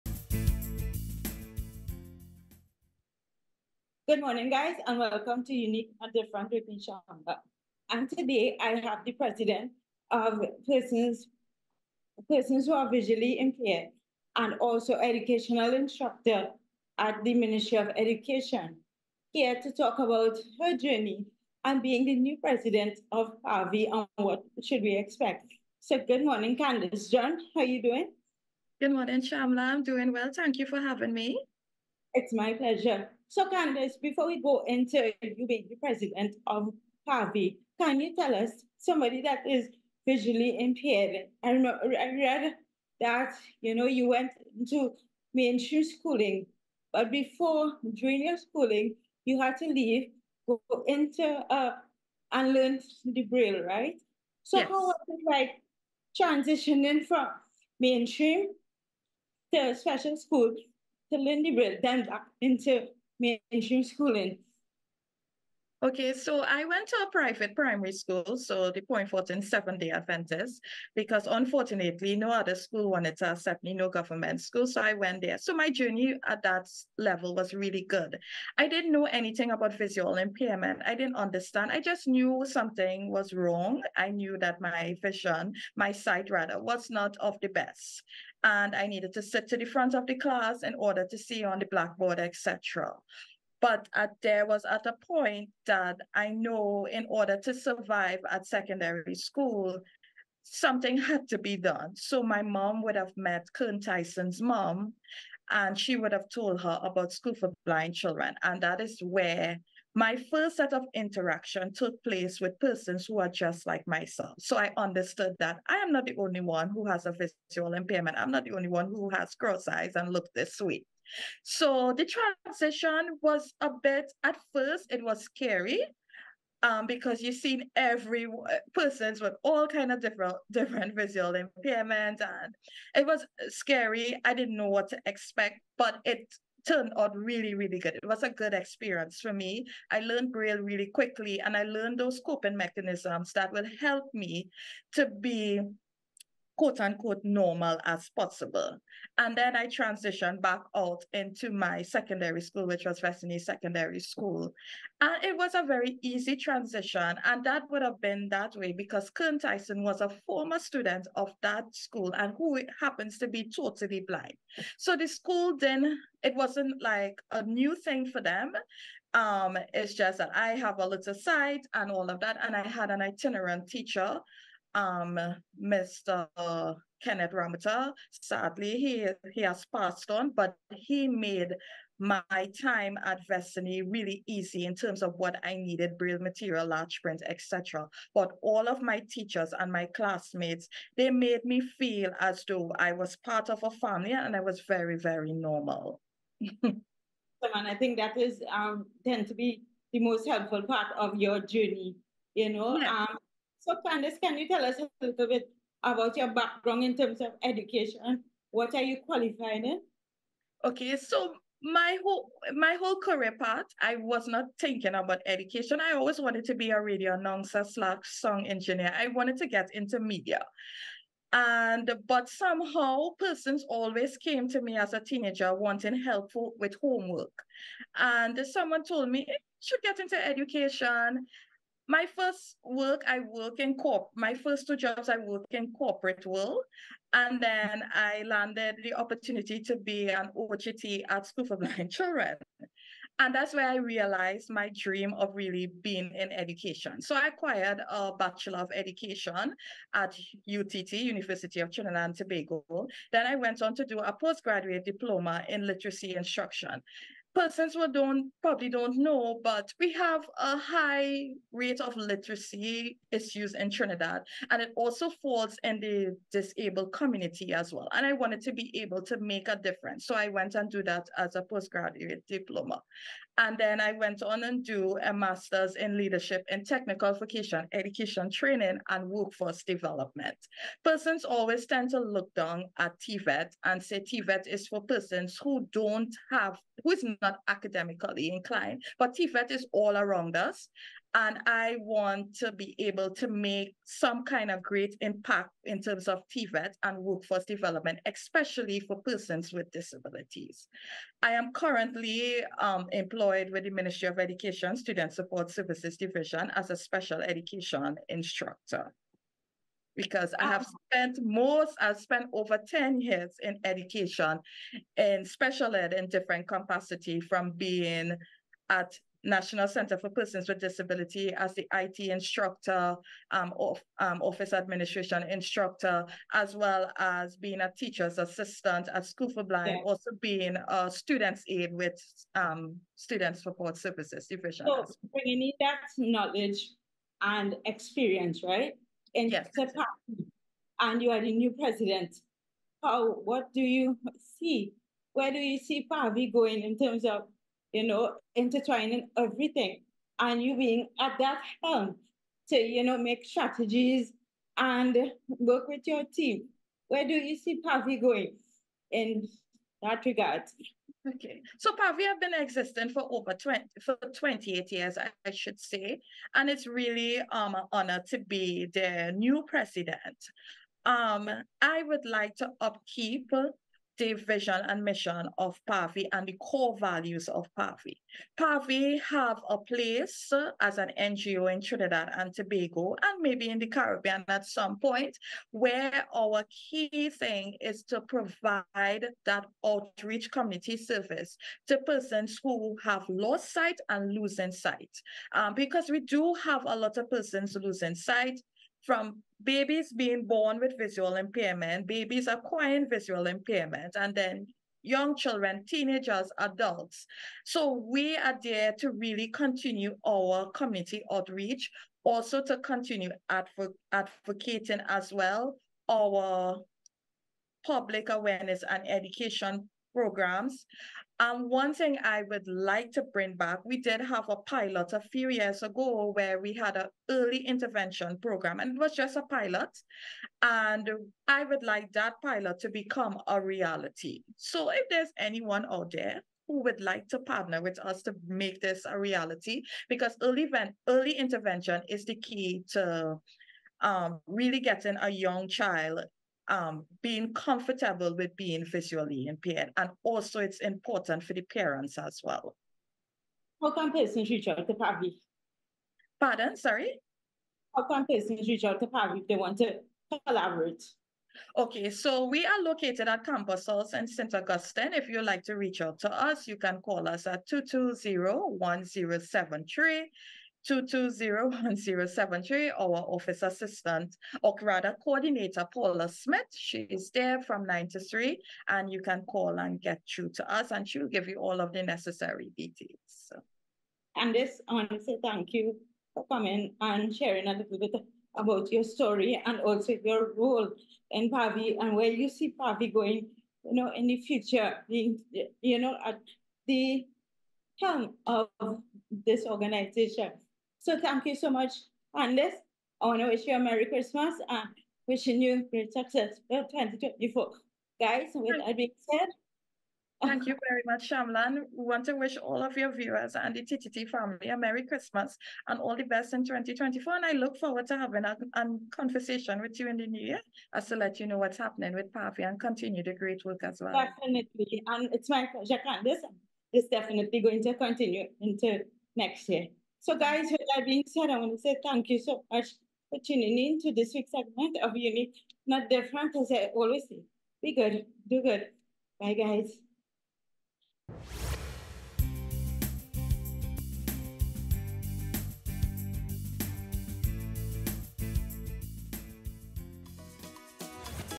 Year End Assessment Of PAVI
In this week's Unique Not Different, Shamla talks with President of Persons Associated With Visual Impairment, Candice John about PAVI's impact & vision for 2024.